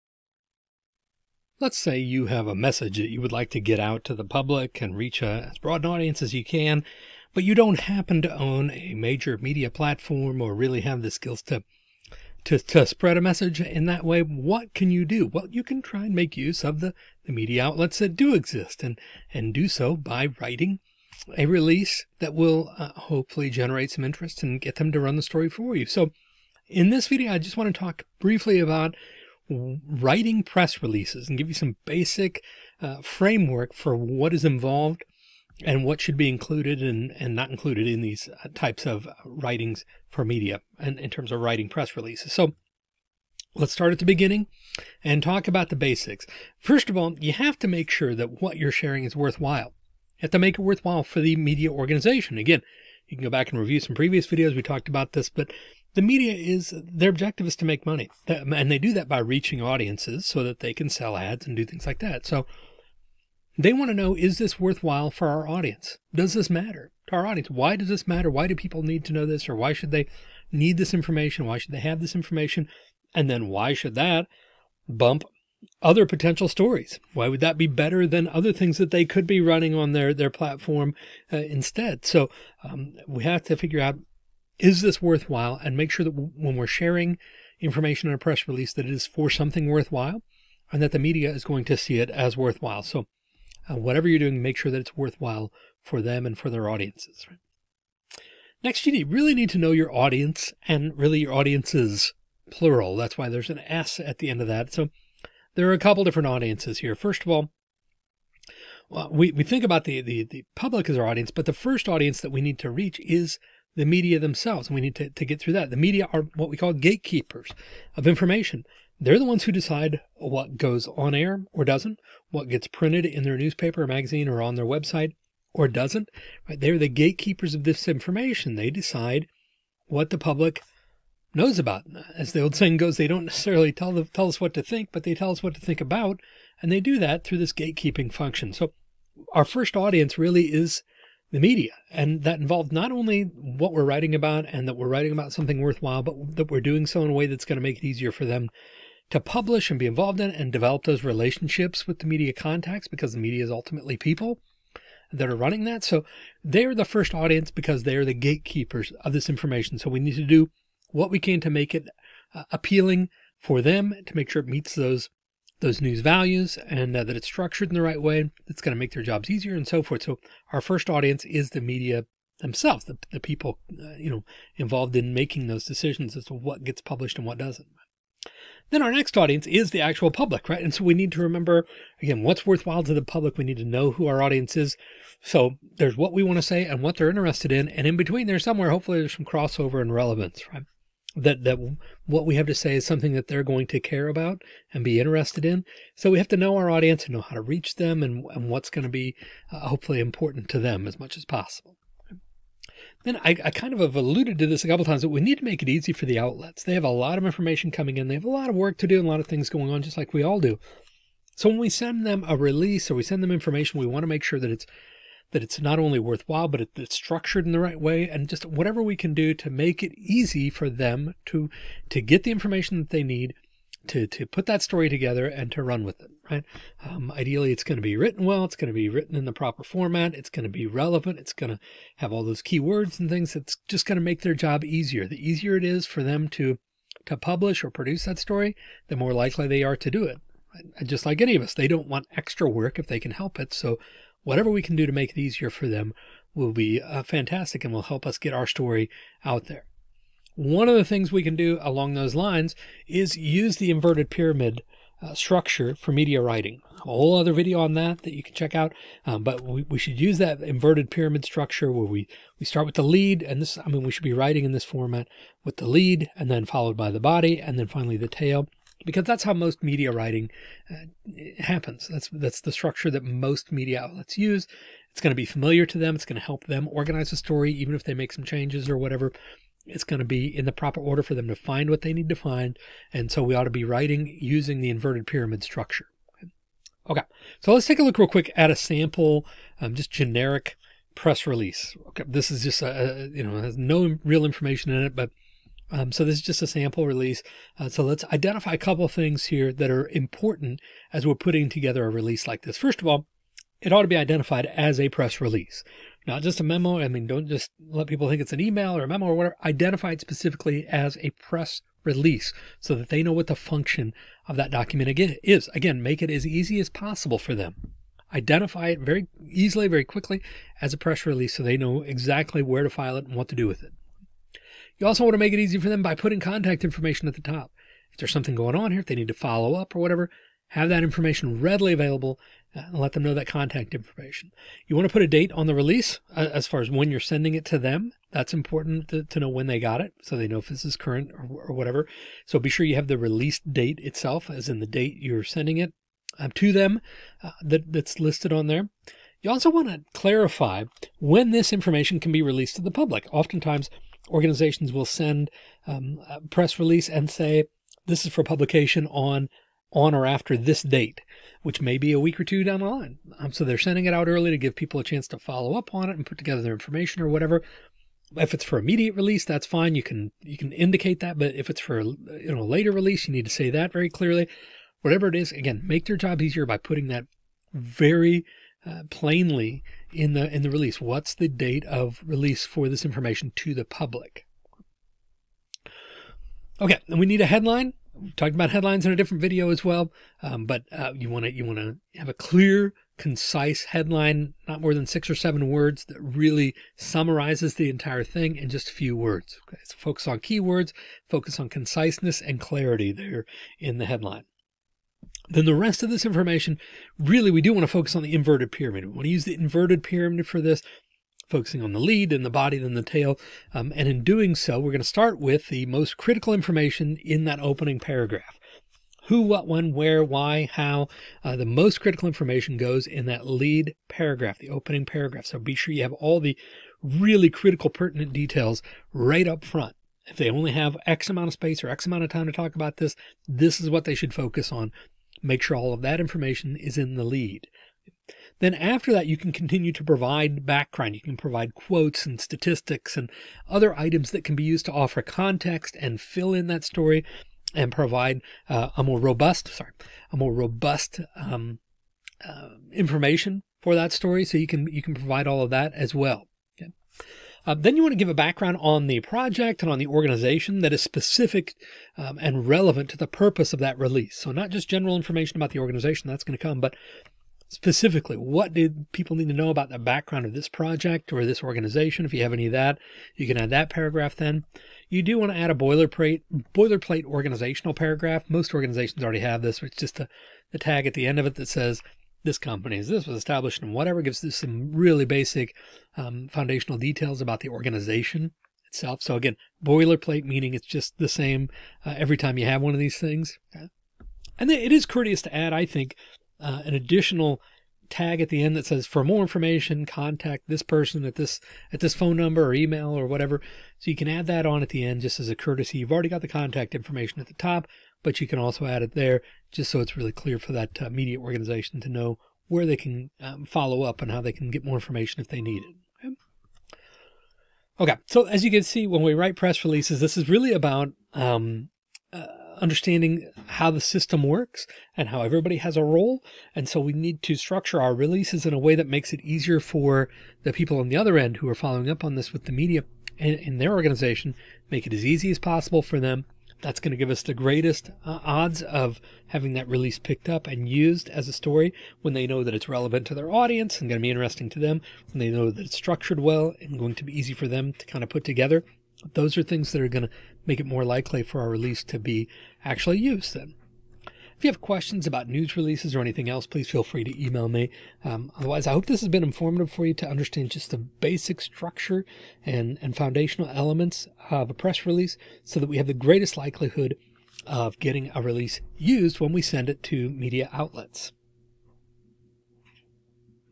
Writing Press Releases
Mini-lecture providing an overview of writing effective press releases with an eye toward making it past media gatekeepers.